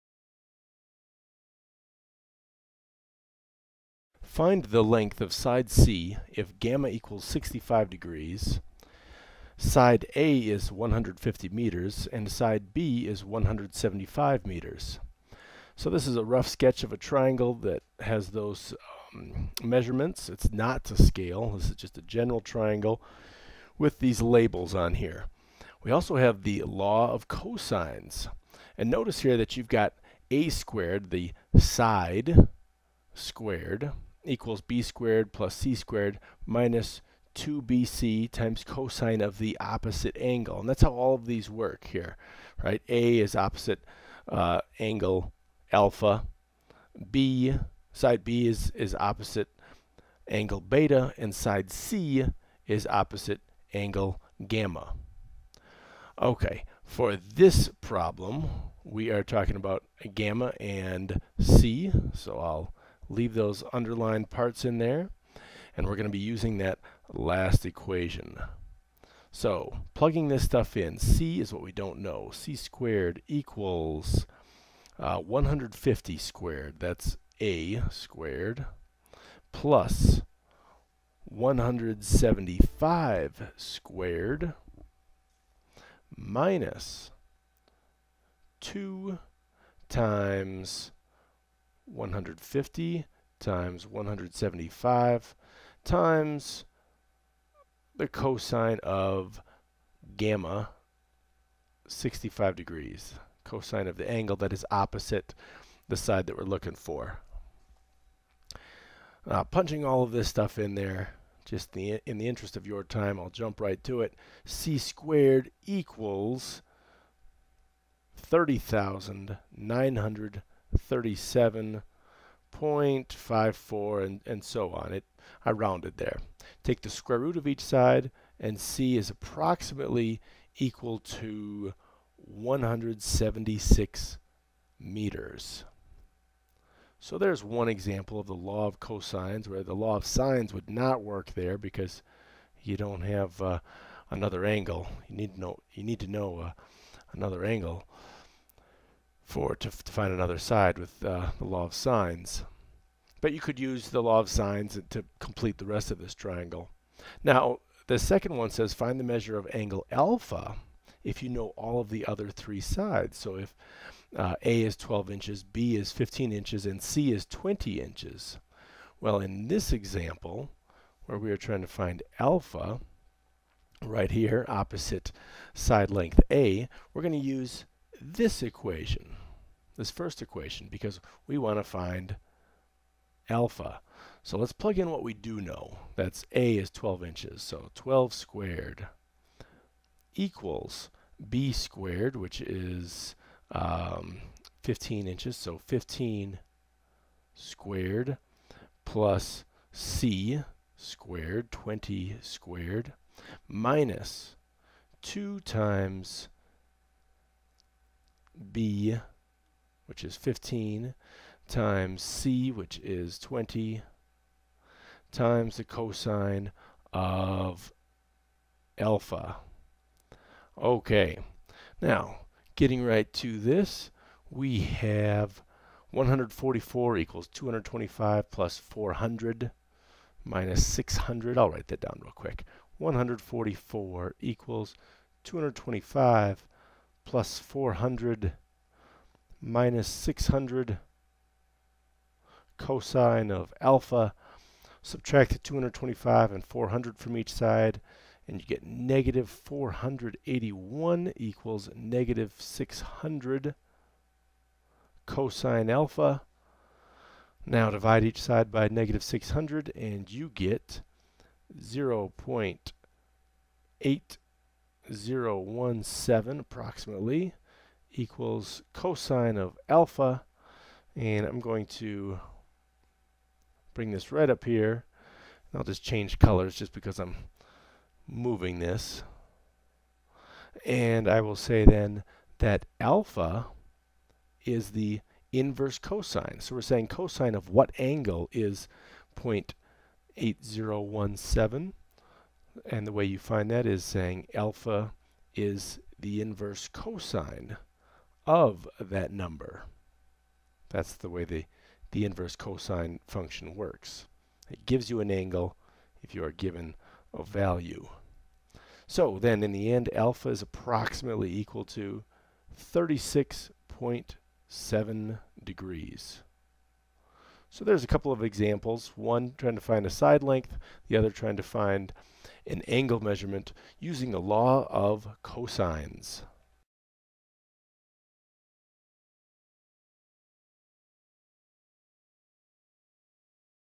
The Law of Cosines - Precalculus Tips
Review The Law of Cosines in this Precalculus tutorial.  Watch and learn now!  Then take an online Precalculus course at StraighterLine for college credit.